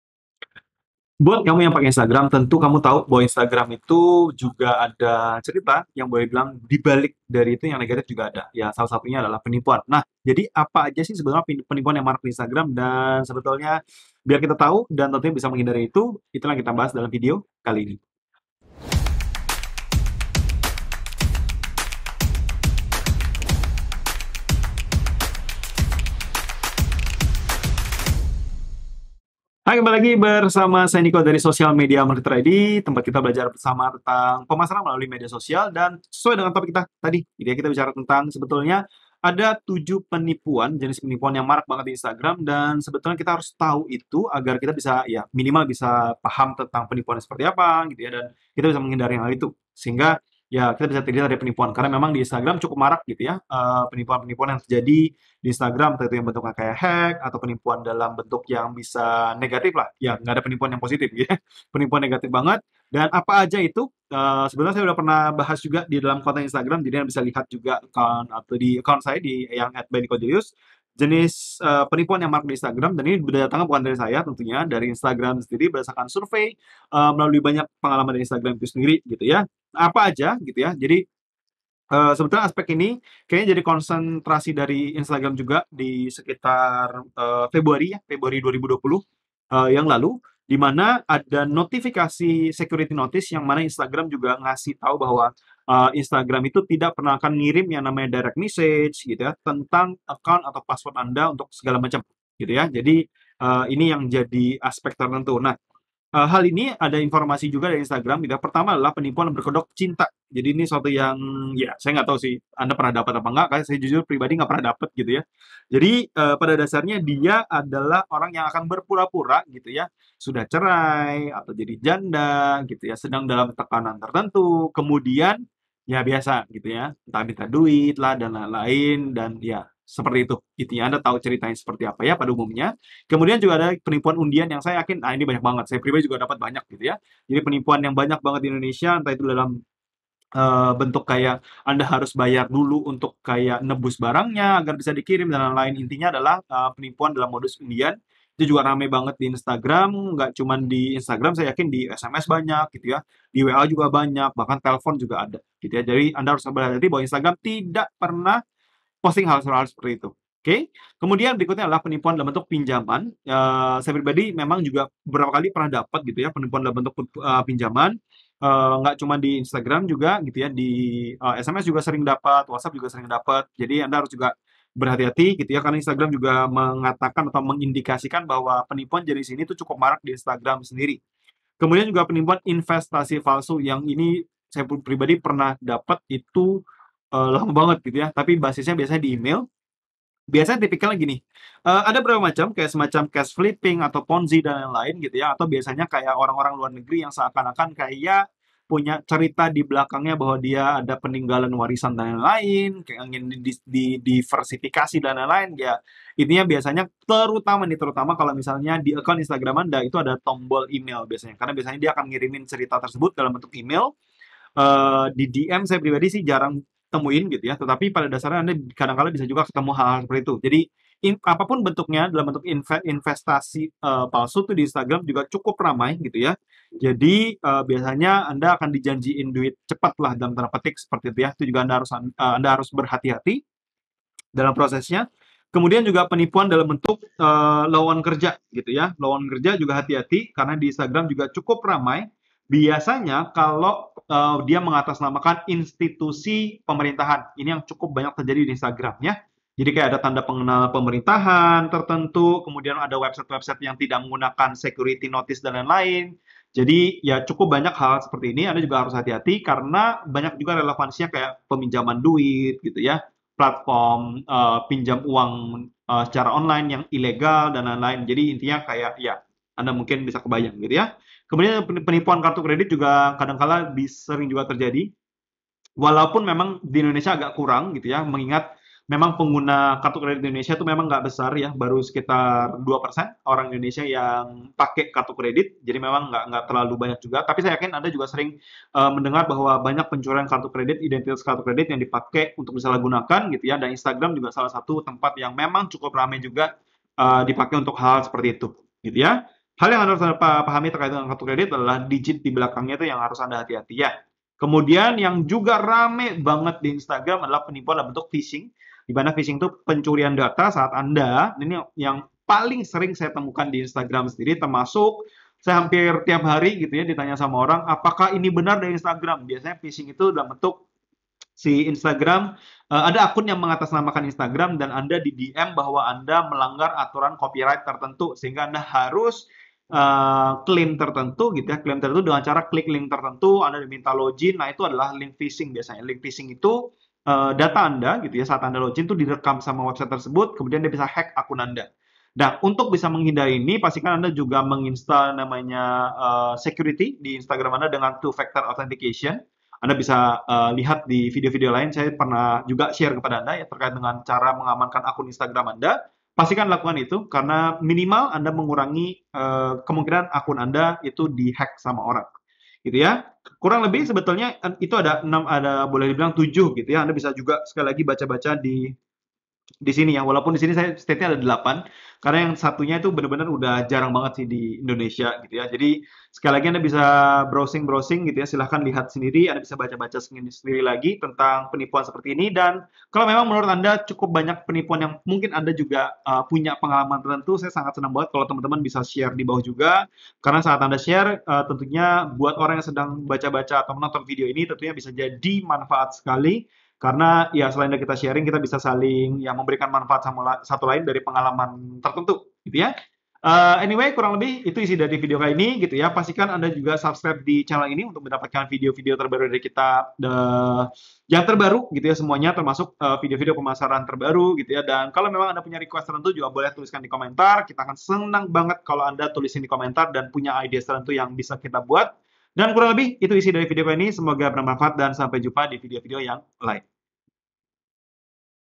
Ini 7 Jenis Penipuan yang Marak di Instagram!
Maraknya penipuan di Instagram tentu menjadi sesuatu yang wajib kita hindari sebisa mungkin. Menariknya, di Indonesia ada beberapa jenis penipuan yang sering sekali dipakai bahkan dibahas oleh Instagram langsung lho...

Tonton videonya ya!

dan dapatkan